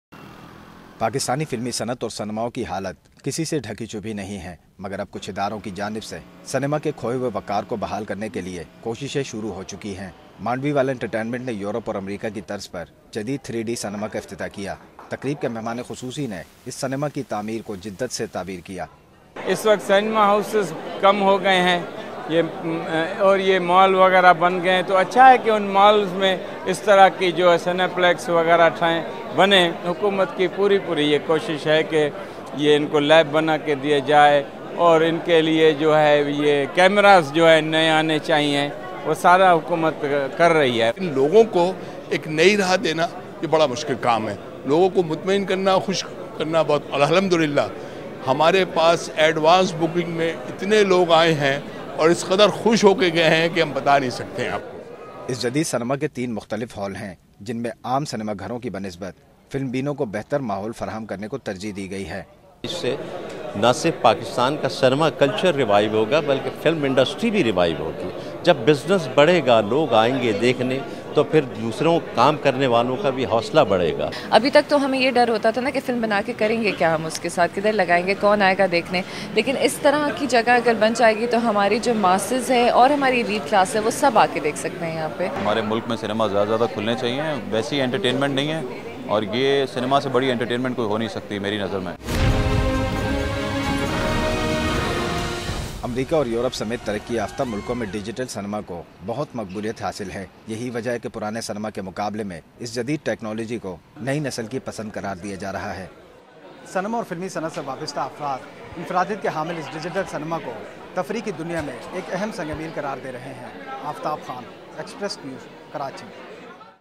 پاکستان کا پہلا ڈیجیٹل تھری ڈی سنیما لانچPakistan's first Digital 3D Cinema Launch
پاکستان کے پہلے ڈیجیٹل تھری سنیما گھروں کا اجراء ، برانڈ ٹی وی کے عاصم قریشی نے منڈوی والا انٹرٹینمنٹ اور ریجنٹ پراپرٹیز کی جانب سے کیا۔ ویڈو بشکریہ ایکسپریس نیوز۔
The Launch of Atrium Cinemas, Pakistans first digital 3d cinema organiser Asim Qureshi of BrandTV on behalf of Mandviwalla Entertainment and Regent Properties.Video courtesy Express News.